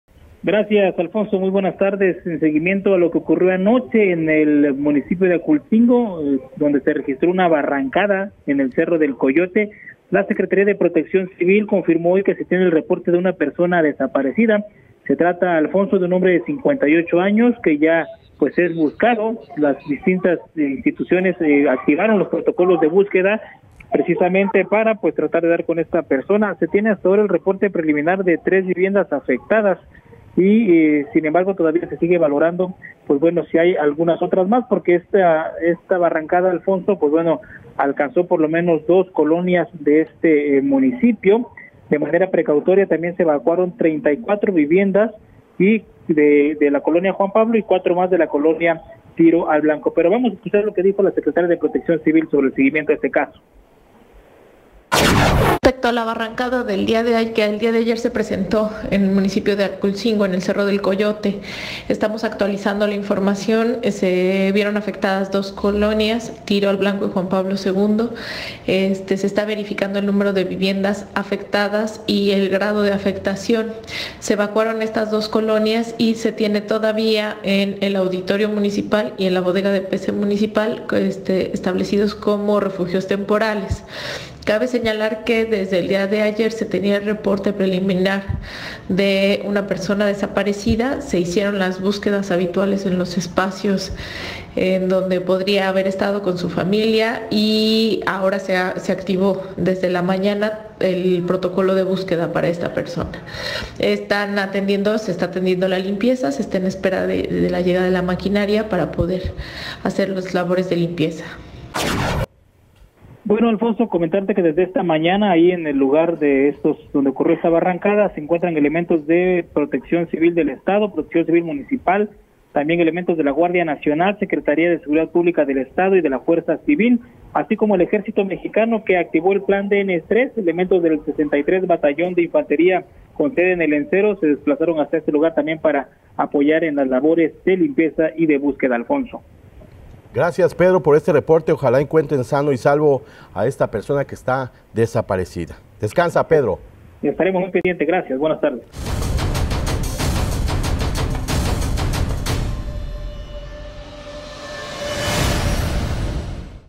Aplica el plan DN-III-E en el municipio de Acultzingo
Personal perteneciente al 63/o. Batallón de infantería, con sede en el Lencero, Veracruz, aplica el plan DN-III-E en el municipio de Acultzingo, por el desgajamiento del cerro del Coyote,  realizando actividades de limpieza y remoción de escombro en apoyo al pueblo de México.